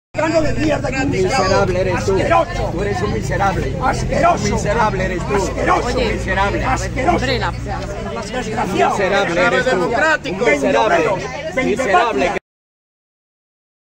Un grupo de “fascistas” increpa a Iglesias y Montero antes del juicio por el acoso a su familia
Fuerte encontronazo a las puertas del Juzgado de lo Penal número 14 de Madrid. El exvicepresidente del Gobierno, Pablo Iglesias, se ha encarado con un grupo de personas que le increpaban a las puertas de la sede judicial, donde se celebra el juicio contra Miguel Ángel Frontera, el vecino de Brunete que está acusado de delitos de acoso, injurias graves con publicidad a una autoridad y descubrimiento de secretos, tras siete meses de “acoso y violencia” al chalet del ex líder de Podemos y de la ex ministra de Igualdad, Irene Montero, así como el resto de su familia.